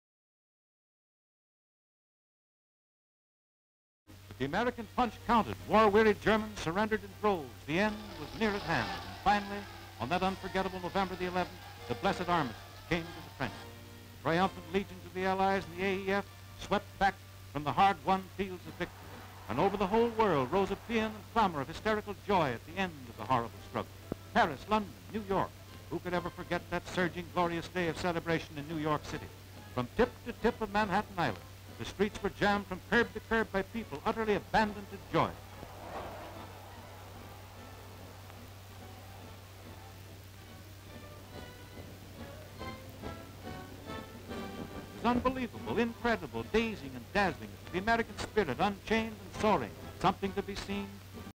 Armistice brings WWI fighting to an end, on November 11, 1918. German troops surr...HD Stock Footage
Armistice brings WWI fighting to an end, on November 11, 1918. German troops surrender. American troops leave combat zones.

Armistice ends hostilities of World War I. German troops surrender and large groups of Germans marching. American flag hoisted to symbolize Allied victory. American troops leave the war zone. People all over the world celebrate the end of war. People gather in New York to celebrate the end of World War I, smiling and waving happily. Location: United States. Date: November 11, 1918.

57,000+ broadcast-quality historic clips for immediate download.